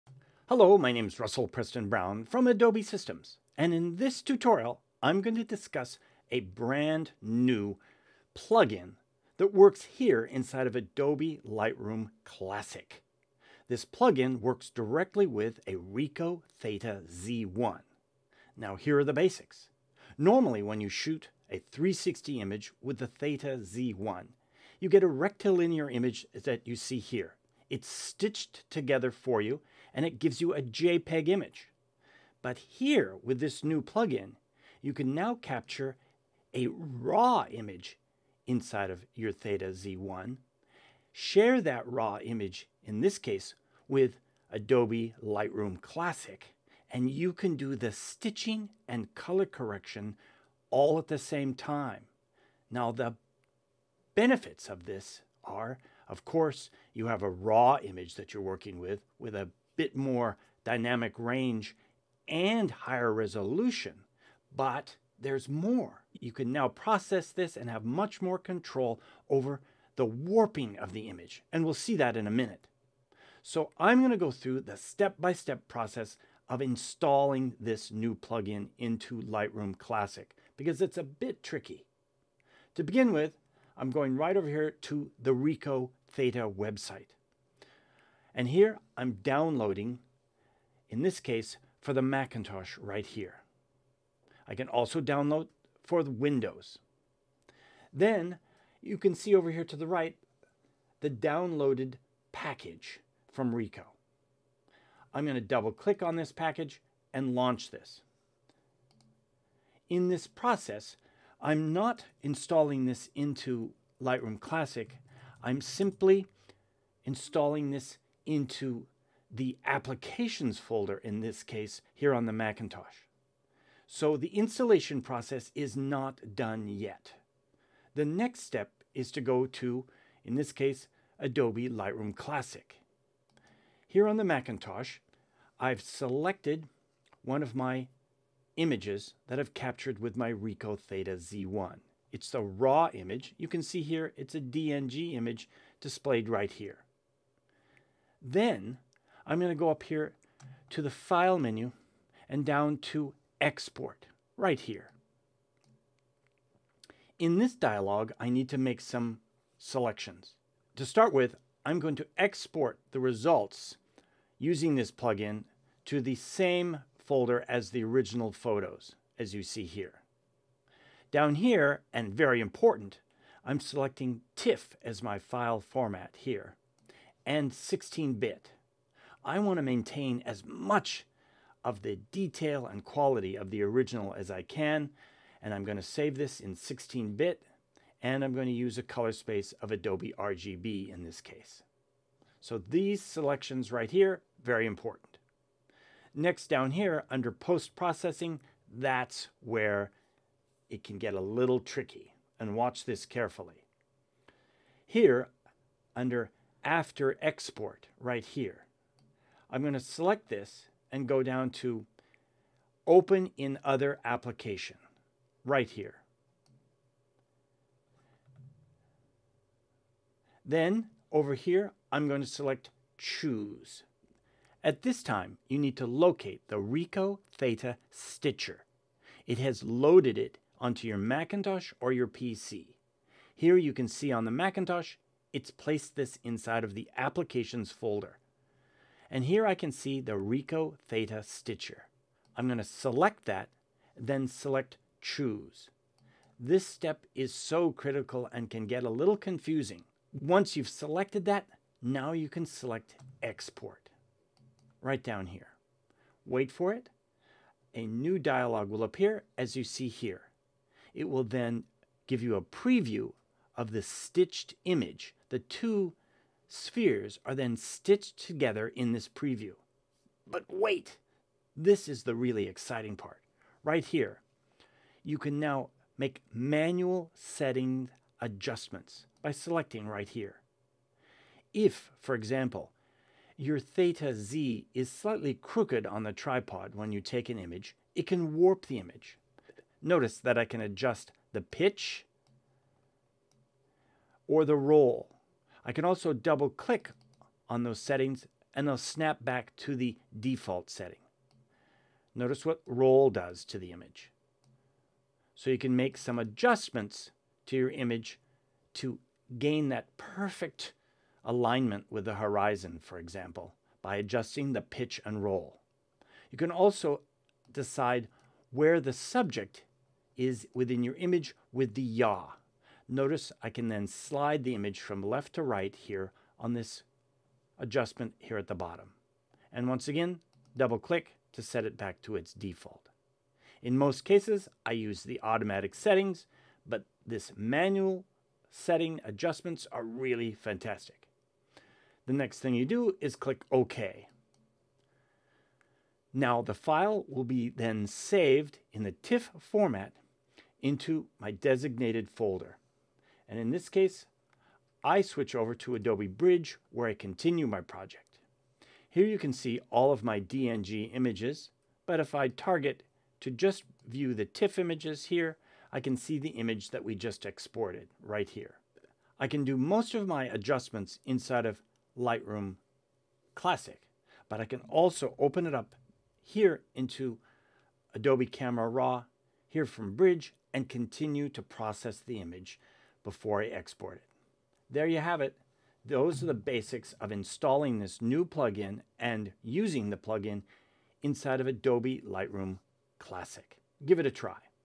THETA Z1 Plug-in Installation Tutorial(Step #1)
RICOH THETA Z1 Plugin for Adobe Lightroom Classic.
This tutorial discusses the installation process for this Lightroom Classic plugin.

Video by Russell Brown